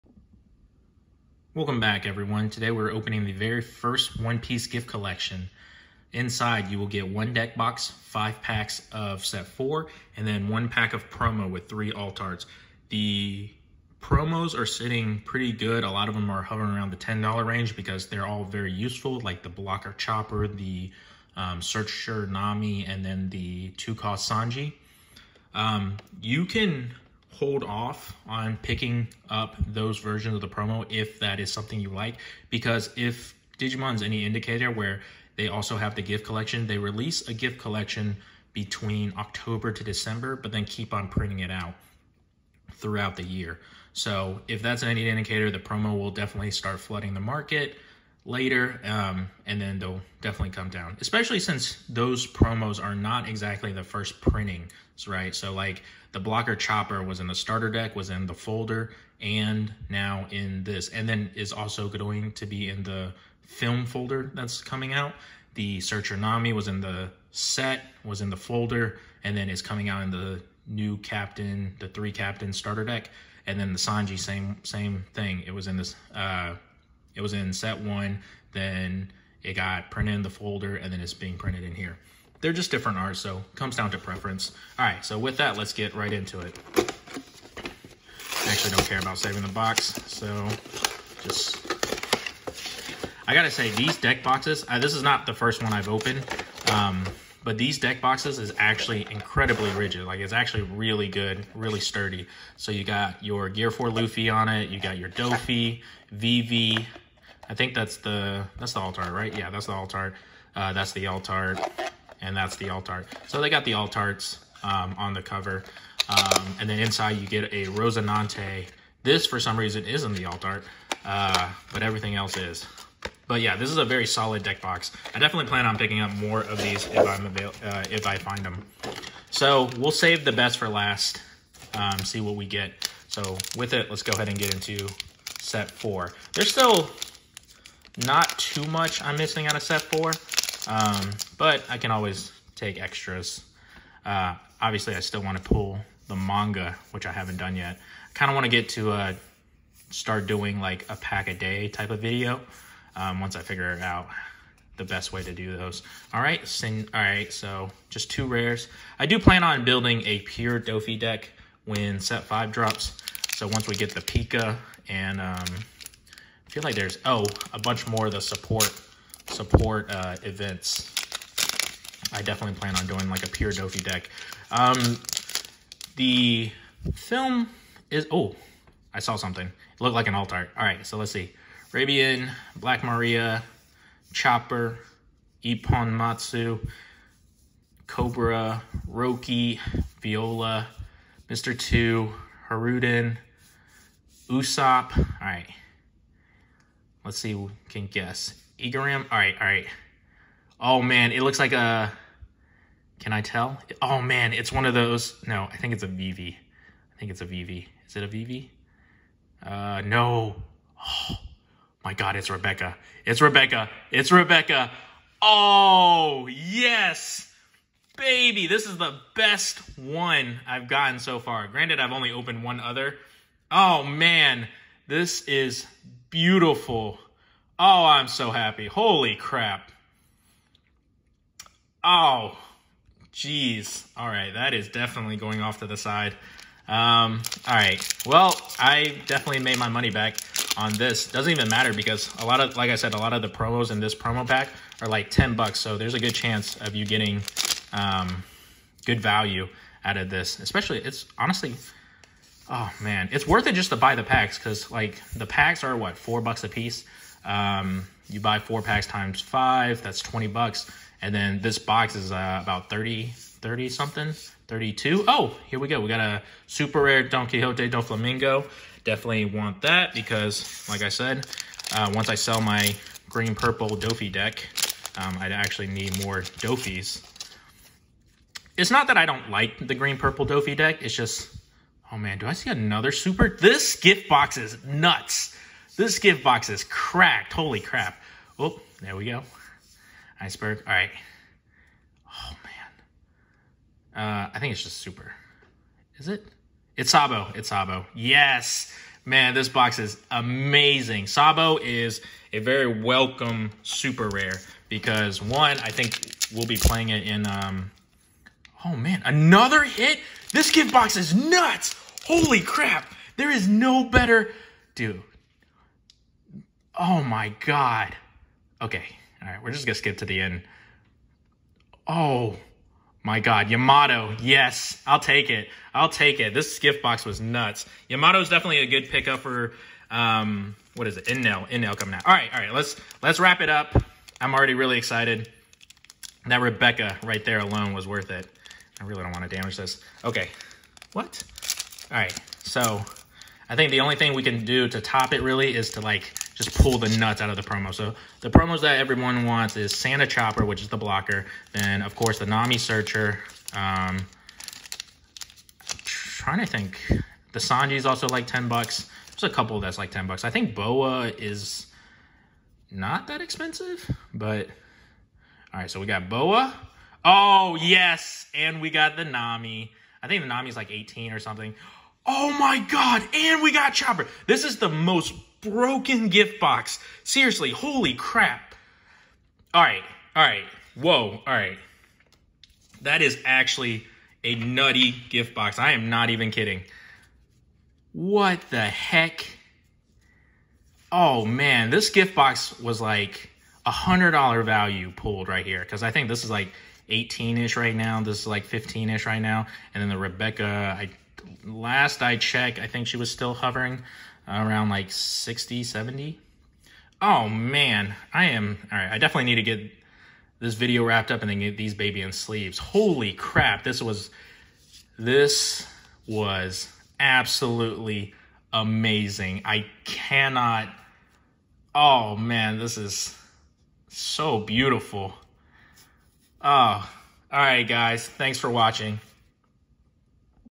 THE BEST ONE PIECE GIFT BOX OPENING!!!
We opened an absolute unit of a gift box!!! Come check it out!